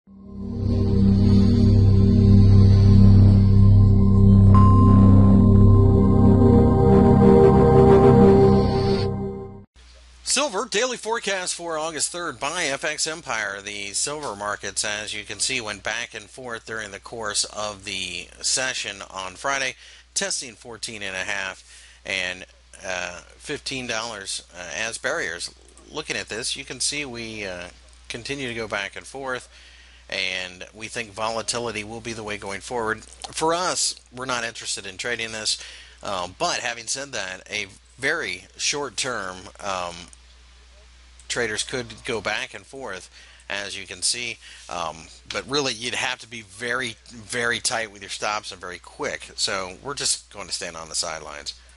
Silver Technical Analysis for August 3 2015 by FXEmpire.com
- August 3 2015 commodity daily technical analysis for the Silver commodity.Find more information about Forex and Commodities News Technical Analysis Fundamental Analysis and Brokers on FX Empire